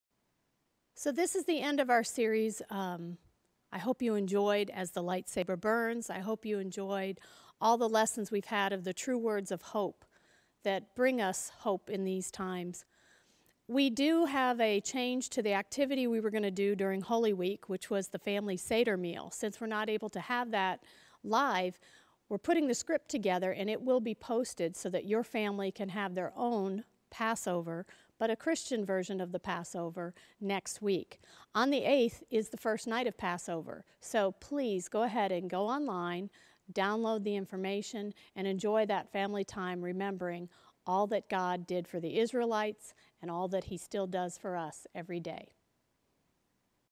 Family Lent 3 5
As you finish the lesson, talk through the questions below, looking for examples in your life where the Words of Hope allow your family to focus on God’s plan during Lent.

1. How was Saul not faithful to what God wants from all His children?
2. What changed from Saul to Paul apart from his name? Which one is considered the “bad” guy vs. the “good” guy? Why?
3. What things did Paul do that showed how faithful he was to what God wanted?
4. How can you be more like Paul? What can you do to be faithful to Jesus?
5. Does being faithful like Paul mean everything in our lives will be easy? What then???

This is part of our Family Lent Service for April 1st, 2020.

If you would like to give as part of your worship experience, just text MLCPLANO to 73256